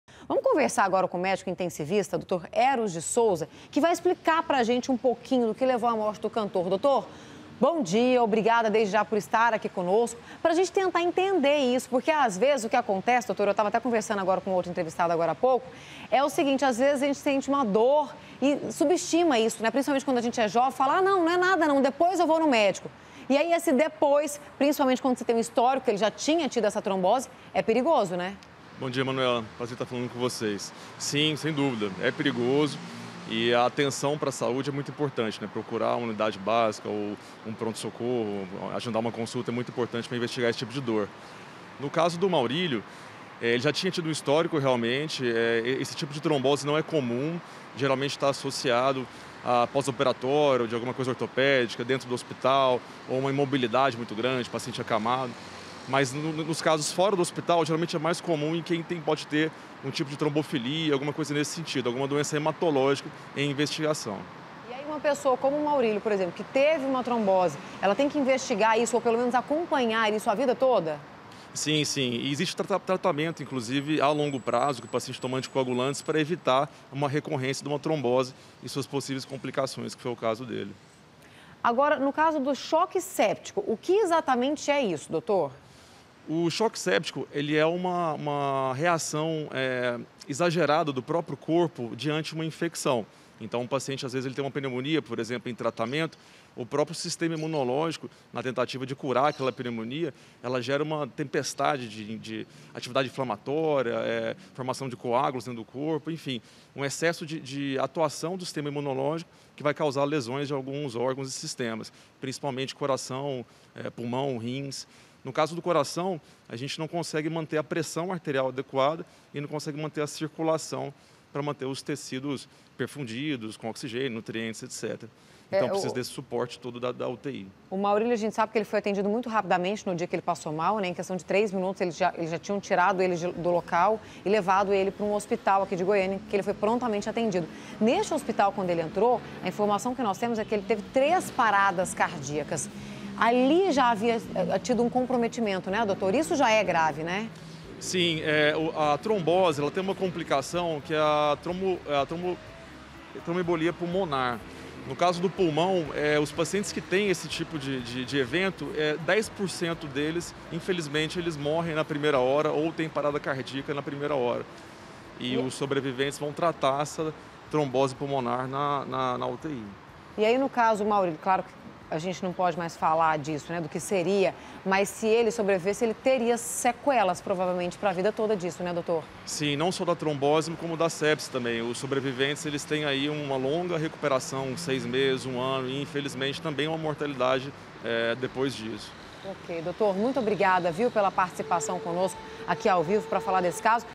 ADEUS A MAURÍLIO: MÉDICO FALA SOBRE PROBLEMA QUE MATOU CANTOR
AO VIVO AQUI COM A GENTE NO GOIÁS NO AR O MÉDICO INTENSIVISTA EROS DE SOUZA QUE PODE EXPLICAR PRA GENTE SOBRE O QUE LEVOU A MORTE DO CANTOR.